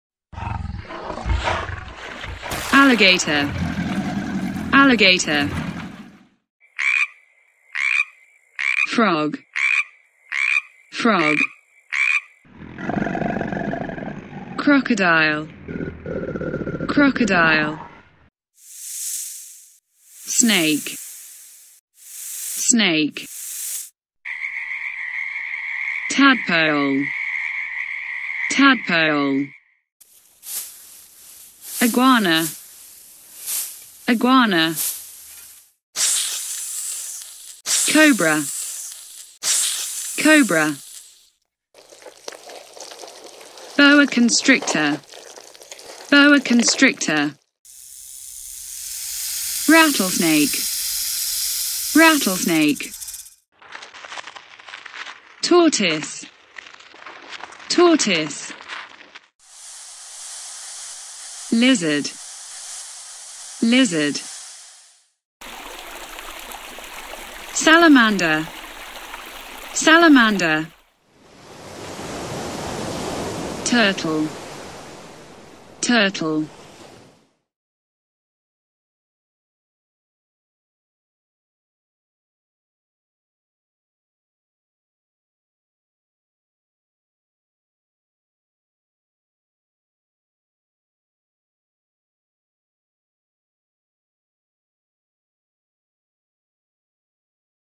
Learn names and Sound Amphibians and reptiles in English for Children
In this video we learn amphibians and reptiles animals in english : alligator, frog, crocodile, snake, tadpole, iguana, cobra, boa constrictor, rattlesnake, tortoise, lizard, salamander, turtle. Fun and creative education video for kids, children, toddlers.